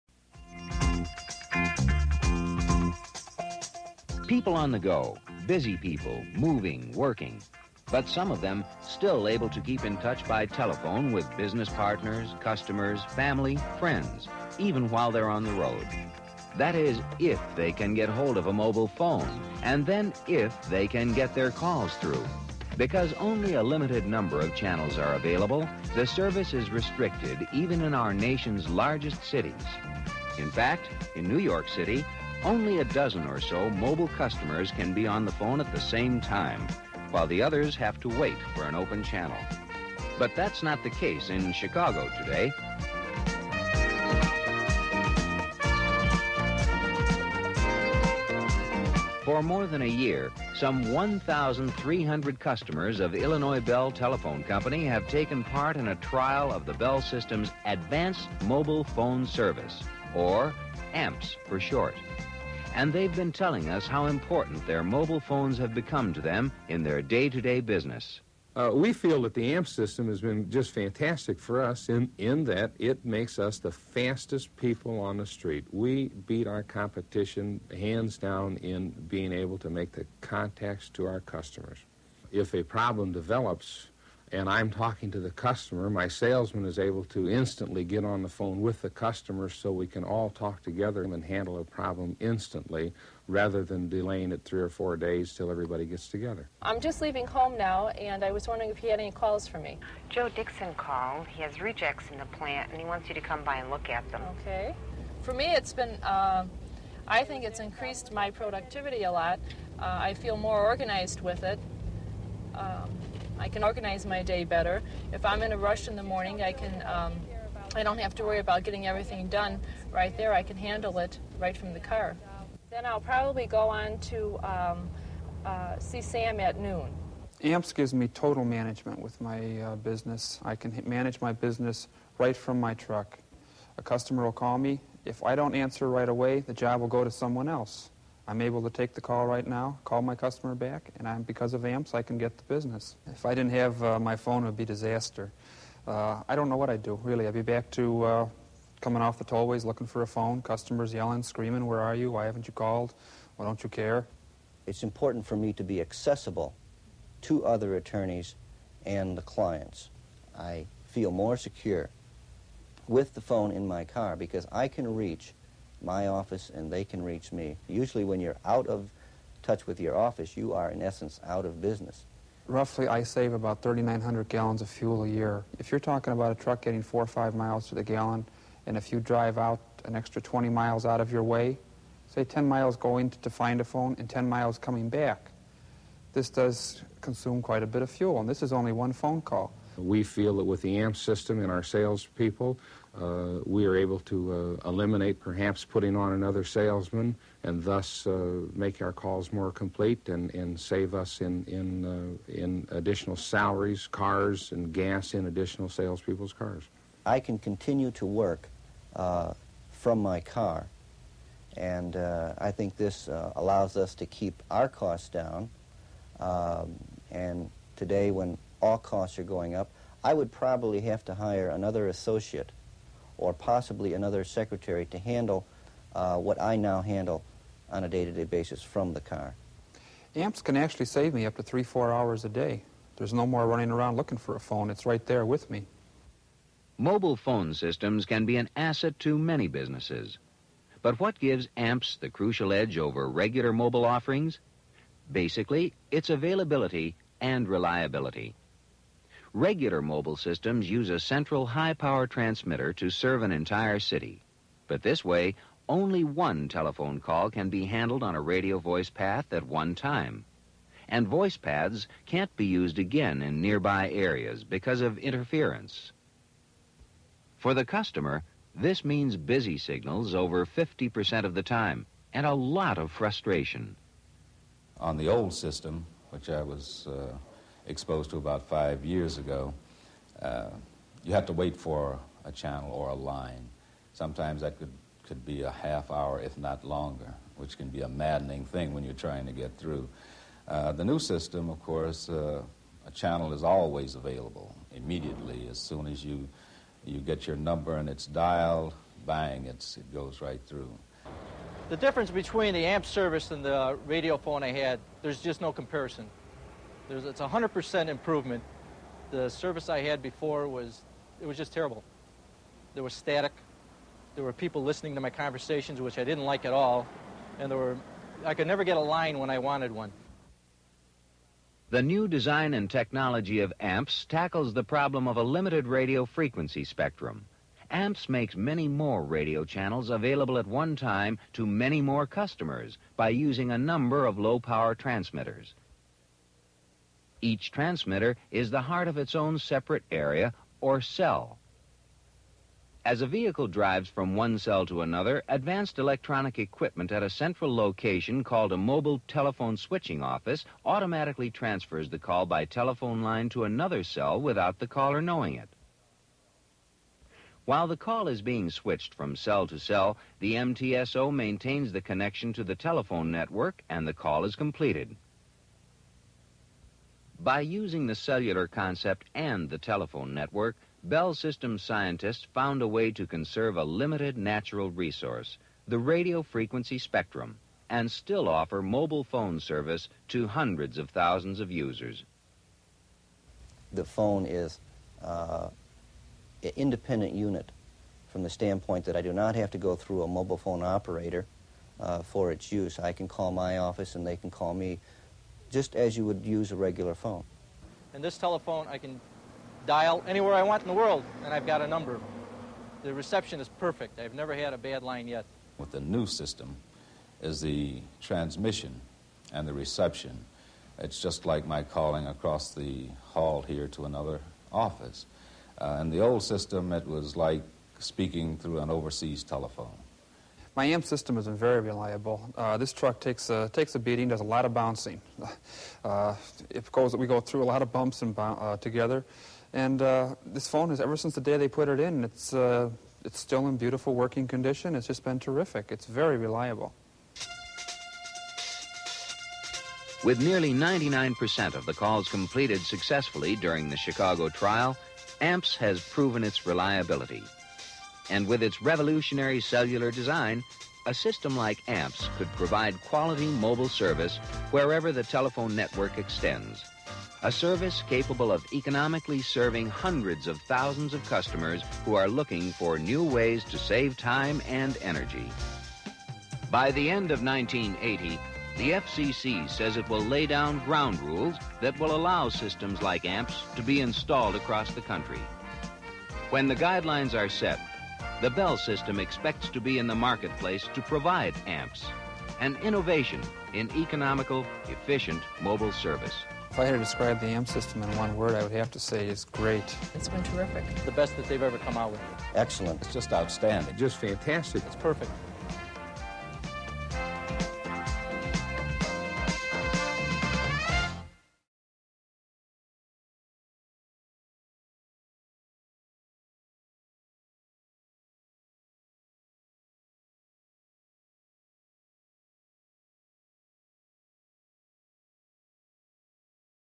AT&T Archives: Testing the First Public Cell Network
Made one year into the first true cell phone trials, this film profiles customer reactions and the technology that made mobile phones possible. The Chicago test cellular network was built in 1977 by the Bell System and had tests during 1978; this film was made in 1979 after a year with 1,300 customers using the system.

The system involved building a group of towers that transmitted telephone calls within their own hexagonal "cell." A call switched from tower to tower as the car went through the cells. The film includes an animated explanation of this. This system is basically still used today, only the network has expanded greatly.

The FCC approved commercial cell phones on a regular basis in 1982. By 1984 there were 25,000 cell phone users, by 2000 that number had ballooned to 100 million.

Footage courtesy of AT&T Archives and History Center, Warren, NJ